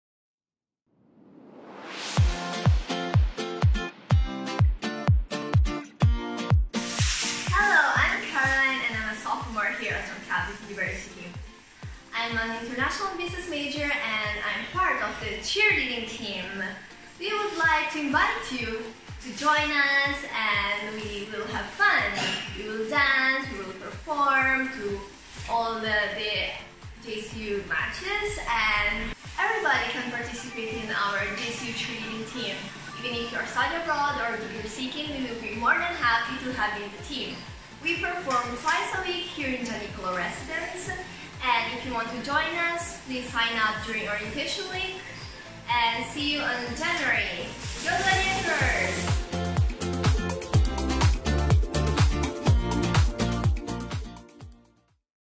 JCU Team Captain Video Interview - Caroline Liquit, Cheerleading
JCU Cheerleading Team Captain Caroline Liquit invites you to join the team in this video-interview!
Let's hear what she has to say!